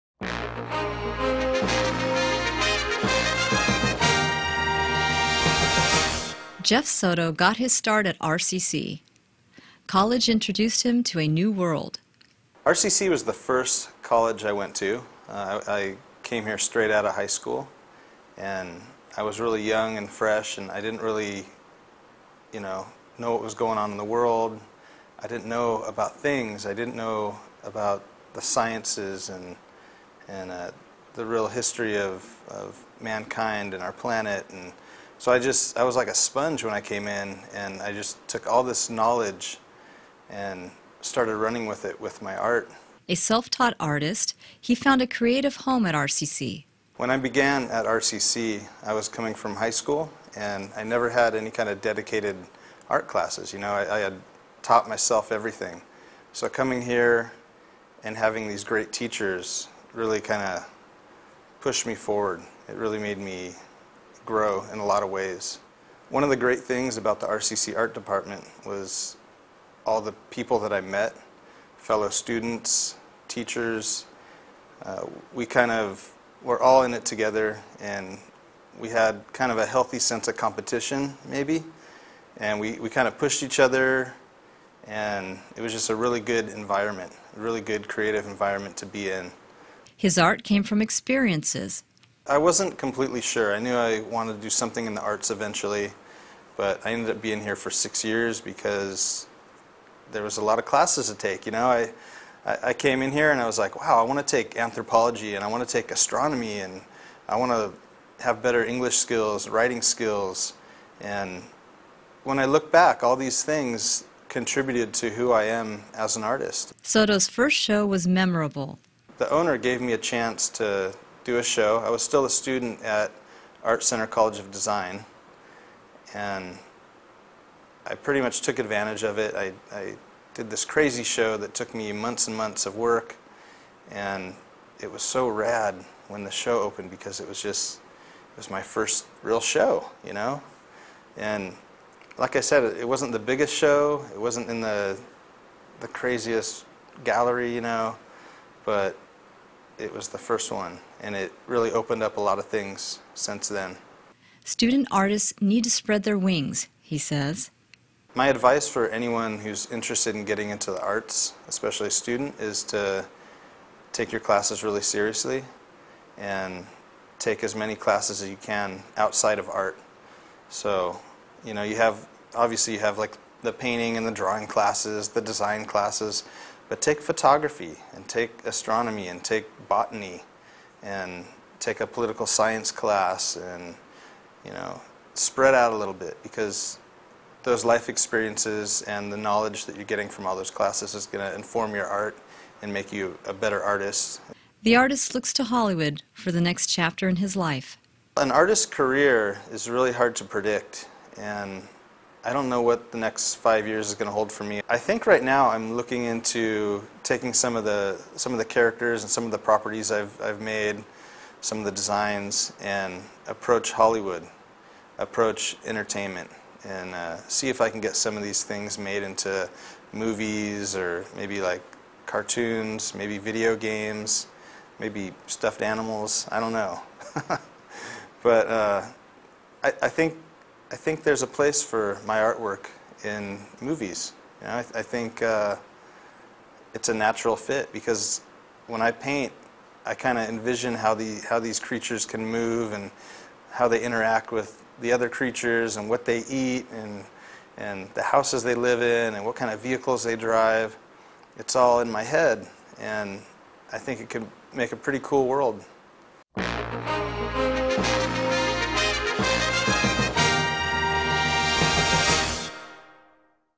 Jeff Soto, painter, illustrator, and muralist
Jeff Soto is a painter, illustrator, and muralist who has exhibited in galleries and museums around the world. As a youth, he simultaneously discovered both traditional painting and illegal graffiti – and, ever since, both worlds have informed his work. The artist’s distinct color palette, subject matter, and technique resonate with a growing audience and bridge the gap between pop surrealism and street art. Inspired by youthful nostalgia, nature, and popular culture, his bold, representational work is simultaneously accessible and stimulating. Now a visiting Assistant Professor of Art, Riverside City College.